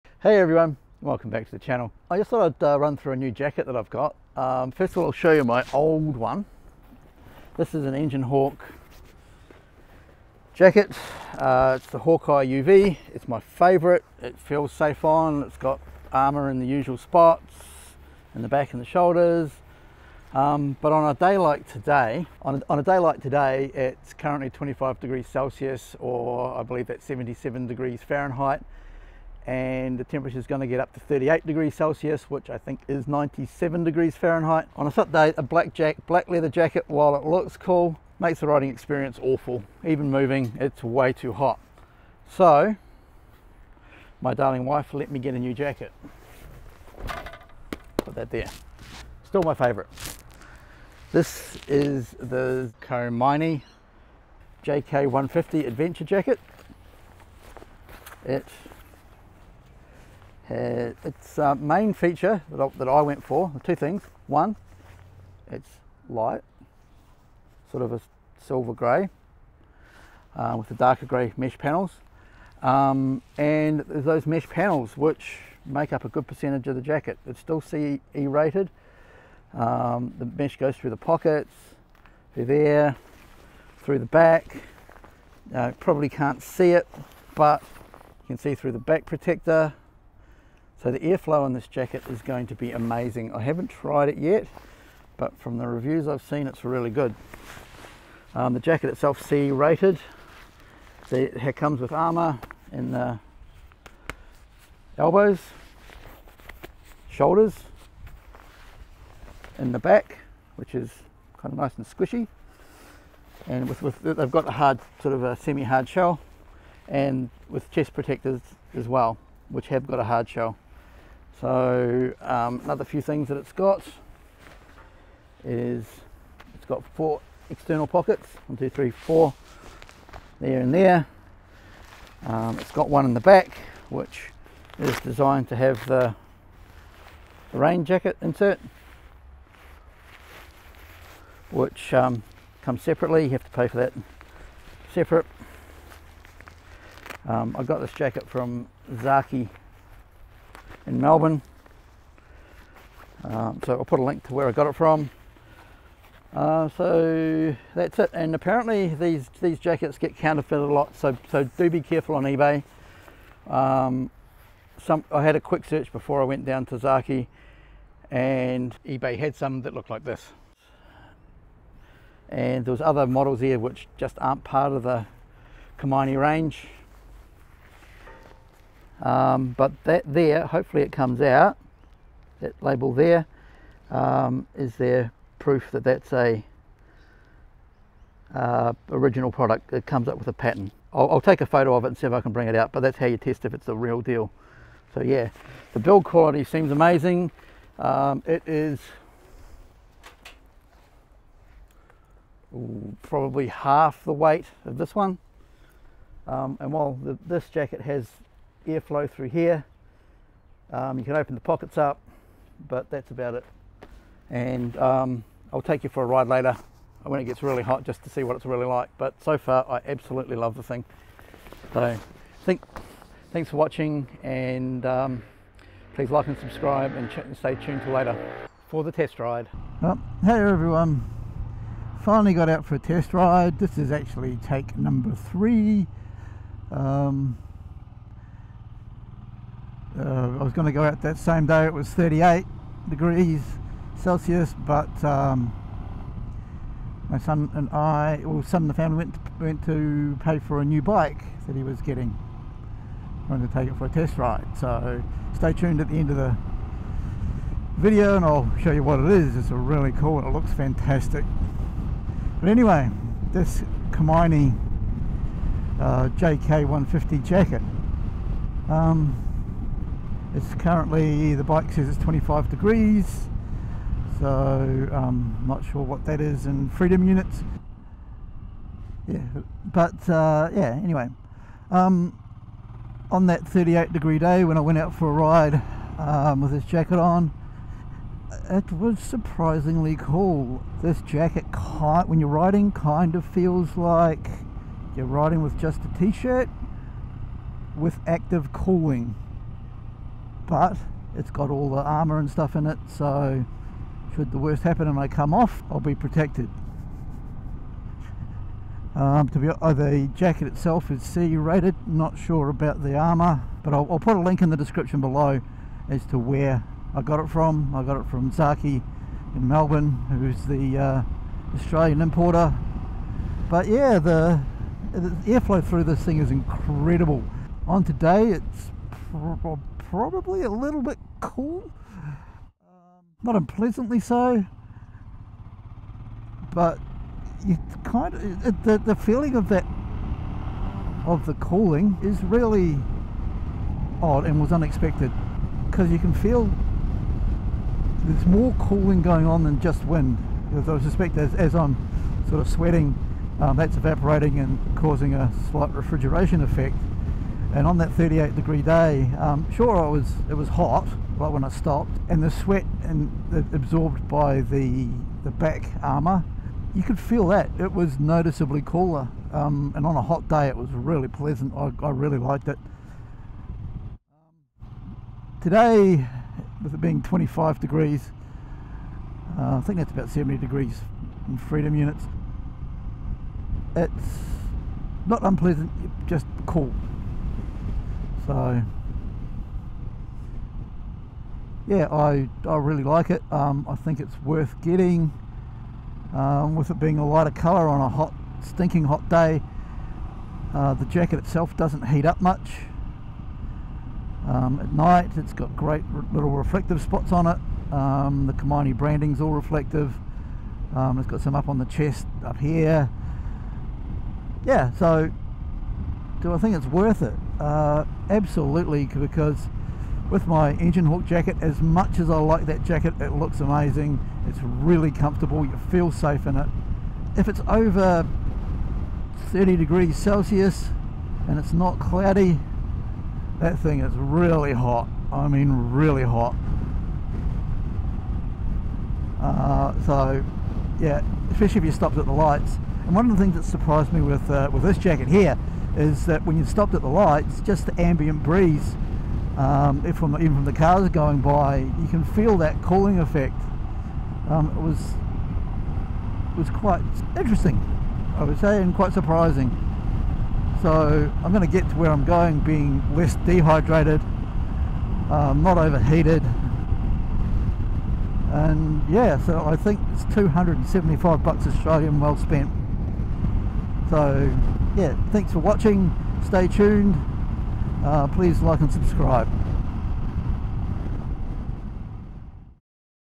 Komine JK 150 Protect Mesh Adventure Jacket Review
This my new Komine Mesh Adventure jacket and what I think of it, while riding my Royal Enfield Euro 5 Himalayan
The Jacket itself is NOT CE rated but its shoulder and elbow armour is.

One thing I didn't say in the video is that the sizing is Japanese and I am in 4xl where my EngineHawk and other jackets are 2XL